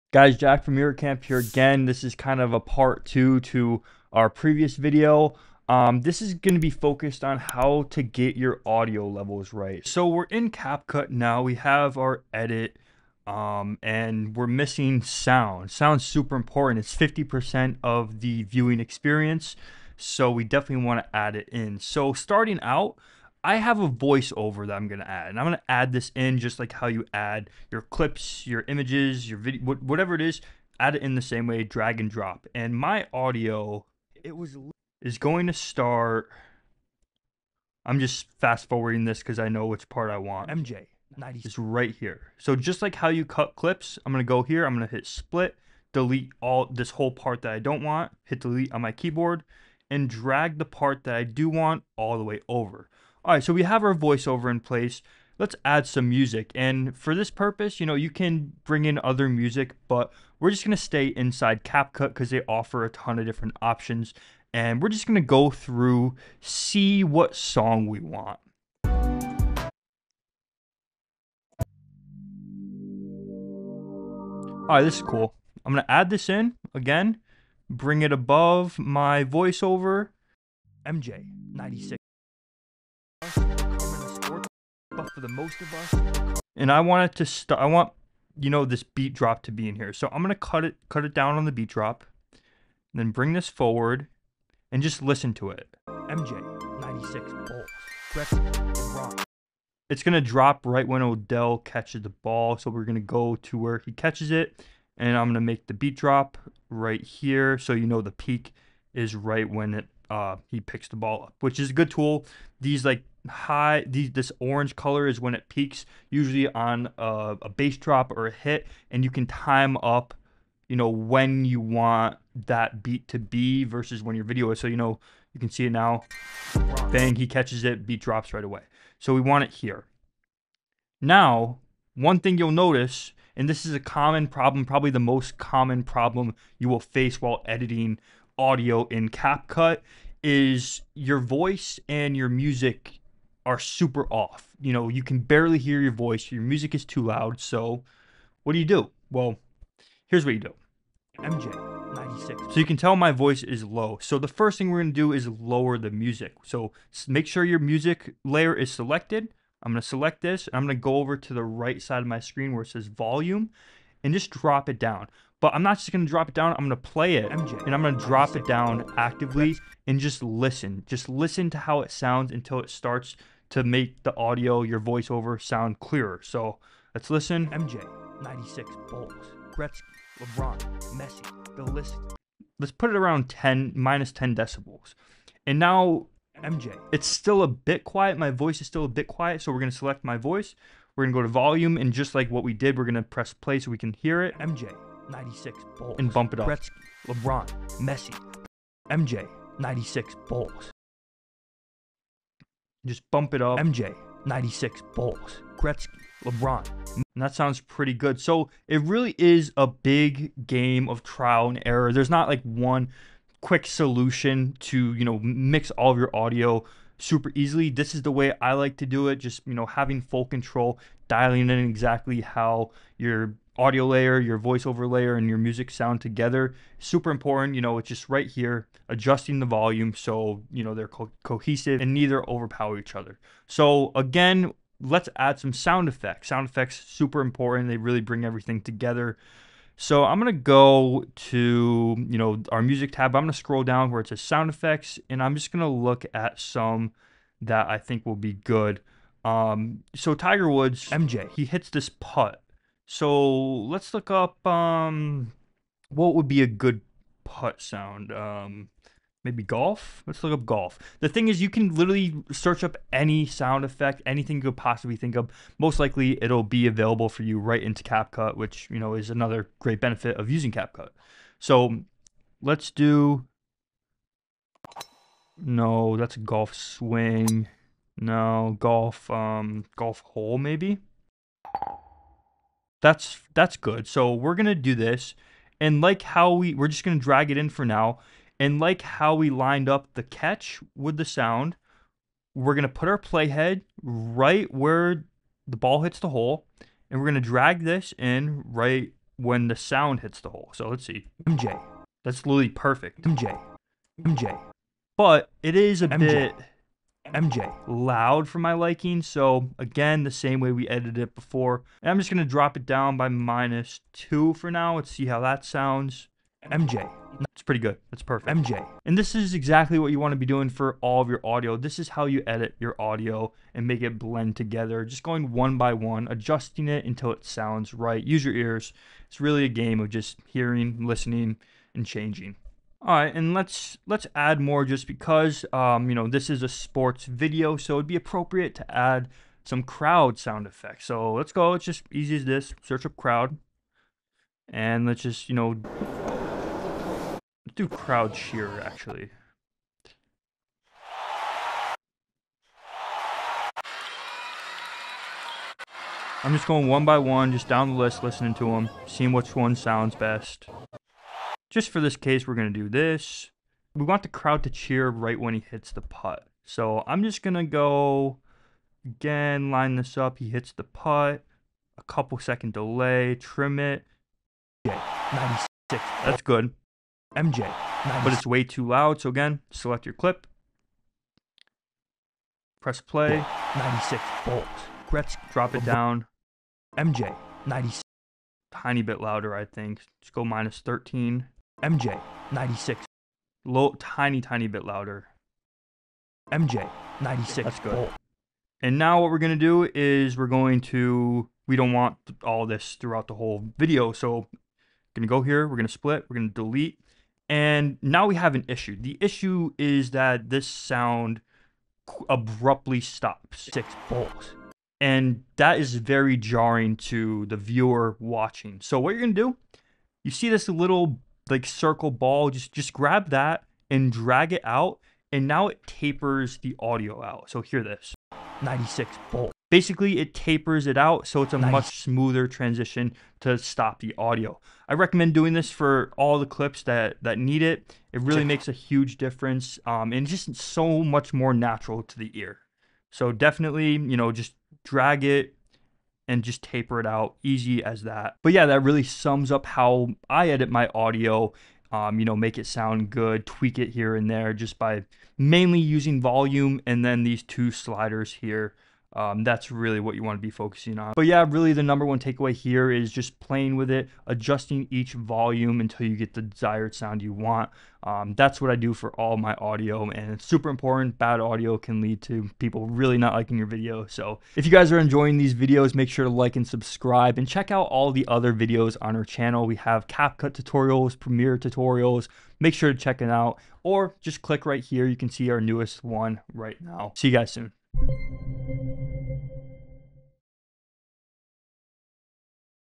CapCut Sound Editing Tutorial Music, Voice-Overs & SFX Explained
Want your videos to sound as good as they look? In this CapCut tutorial, we’ll walk you through everything you need to know to master CapCut sound editing—perfect for beginners and aspiring content creators!

In this video, you’ll learn how to:
✔️ Adjust audio levels in CapCut
✔️ Add professional voice-overs to your projects
✔️ Use CapCut sound effects (SFX) for extra impact
✔️ Sync your video footage with music smoothly
✔️ Create smooth audio fade-ins and fade-outs

Whether you’re working on YouTube videos, TikToks, or short-form content, this tutorial will help you take your CapCut sound video editing skills to the next level.